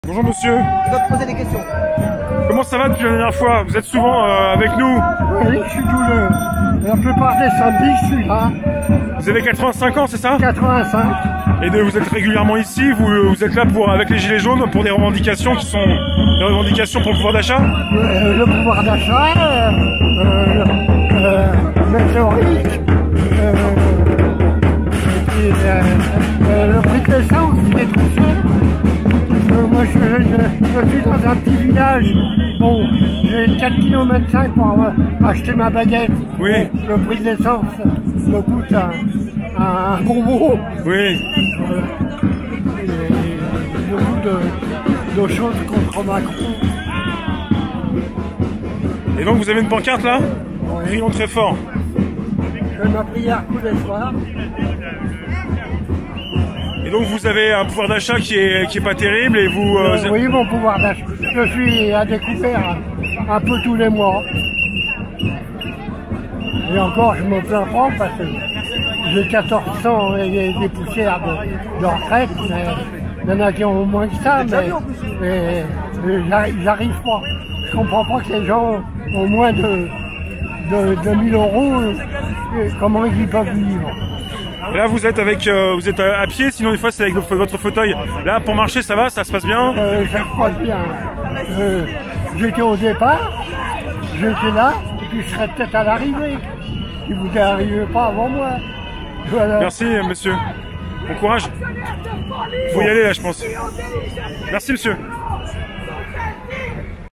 «Le prix de l’essence, il est trop cher. Je suis dans un petit village et je fais 4,5km pour.... »
«Le prix de l’essence, il est trop cher. Je suis dans un petit village et je fais 4,5km pour acheter ma baguette»